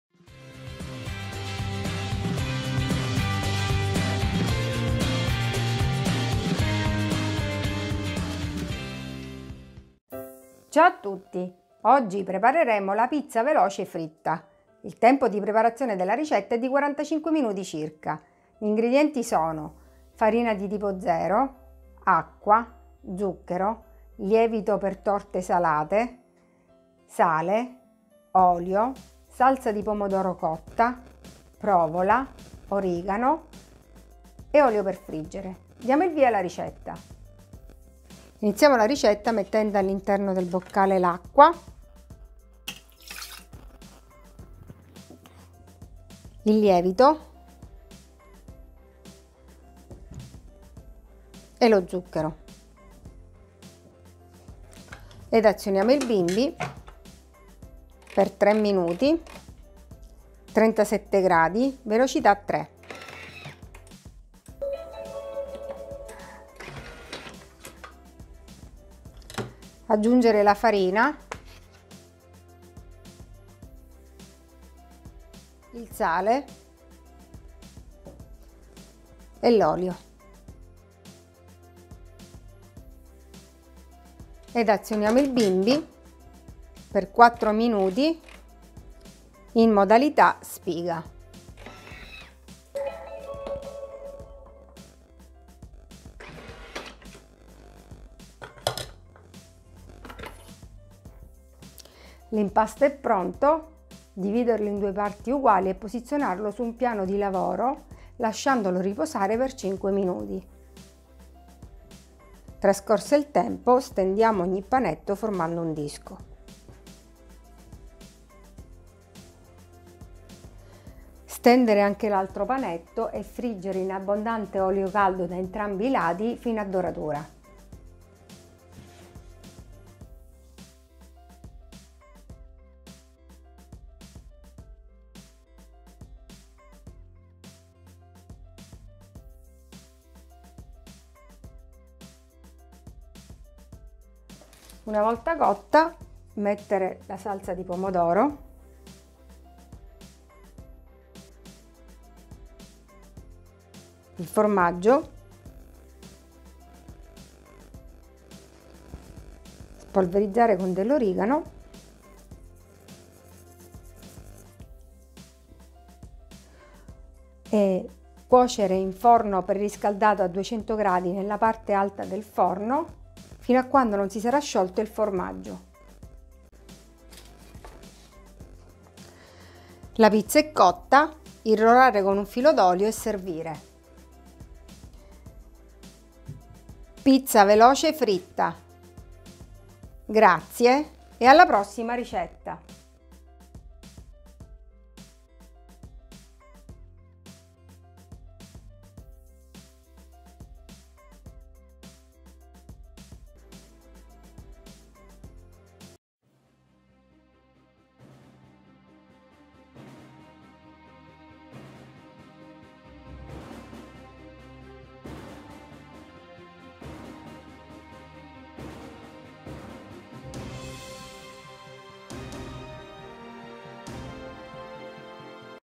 Pizza veloce fritta bimby TM6 TM5 TM31 TM21
Dosi per 2 pizze

Ingredienti per l'impasto:
600 g di farina di tipo "0"
300 g di acqua
2 cucchiaini di zucchero
1 bustina di lievito istantaneo per torte salate
15 g di sale
25 g di olio e.v.o.

Ingredienti per farcire:
200 g di salsa di pomodoro cotta
200 g di provola dolce
origano q.b.